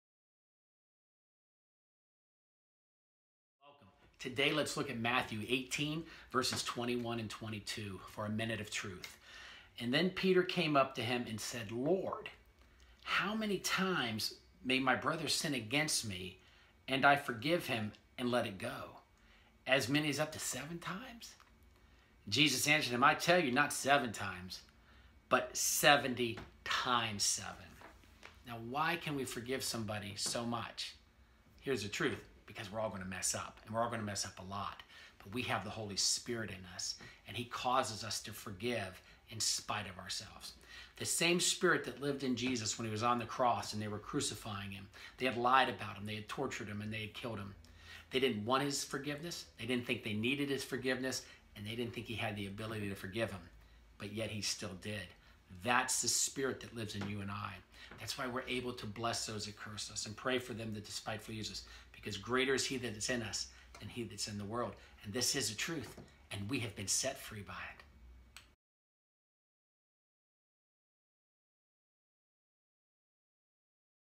1 Minute Of Truth: Jesus Will Cause You To Forgive
An Inspirational Message That Jesus Makes Sure That We Forgive Others